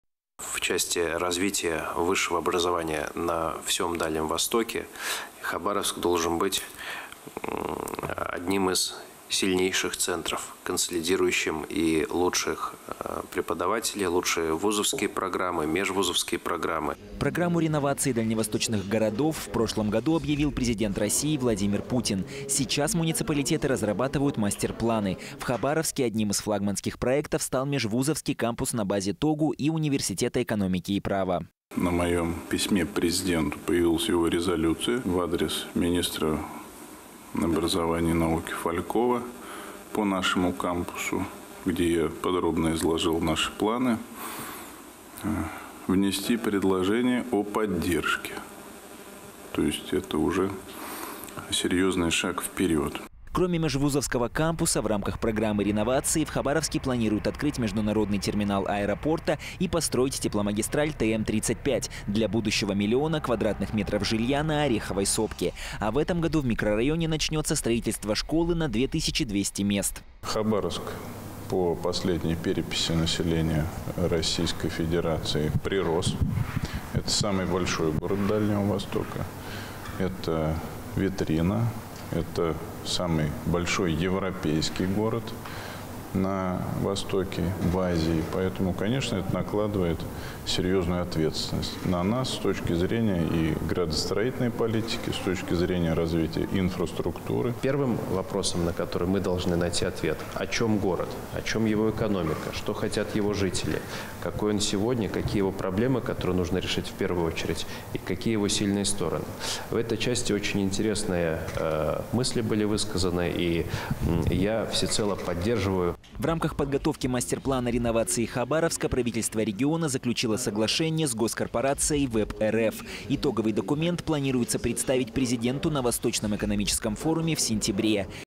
Хабаровск — самый большой европейский город на востоке в Азии / Михаил Дегтярёв
«Хабаровск по последней переписи населения РФ прирос. Это самый большой город Дальнего Востока. Это витрина, это самый большой европейский город на востоке в Азии. Поэтому, конечно, это накладывает серьёзную ответственность на нас с точки зрения и градостроительной политики, и развития инфраструктуры» — Михаил Дегтярёв.

Министр по развитию Дальнего Востока и Арктики Алексей Чекунков, находясь с рабочей поездкой в Хабаровске, провёл ряд совещаний с руководителями органов власти, научно-образовательных учреждений, представителями бизнеса и общественных объединений.

Пресс-служба краевых властей сообщает, что обновление городов Дальнего Востока — один из самых актуальных вопросов федеральной повестки. В крае под глобальную реновацию попадают три административных центра: Хабаровск, Комсомольск-на-Амуре и Советская Гавань. В разработке планов развития городов есть три важных аспекта.

«Во-первых, стратегический, описывающий основные аспекты экономики, смыслы города, идею. У каждого города есть свой характер, миссия, легенды. Во-вторых, урбанистический аспект. Это мастер-планирование, описывающее в архитектурных терминах, что, где и когда должно быть построено. Третий аспект — маркетинговый. Необходимо задействовать все современные маркетинговые инструменты, чтобы обеспечить коммуникацию с жителями, проекты развития должны объединять людей», — сказал министр по развитию Дальнего Востока и Арктики Алексей Чекунков.

Такой подход к долгосрочному социально-экономическому и пространственному развитию территорий был детально рассмотрен на примере Хабаровска.

«По результатам последней переписи населения, Хабаровск признан самым крупным городом на Дальнем Востоке. Сегодня, в рамках подготовки мастер-плана краевой столицы, мы заключили соглашение с госкорпорацией ВЭБ.РФ. Благодаря чему планируем представить итоговый документ президенту России на ВЭФ-2022», — сообщил Михаил Дегтярёв.

Одним из ключевых направлений масштабной модернизации краевой столицы может стать строительство межвузовского кампуса.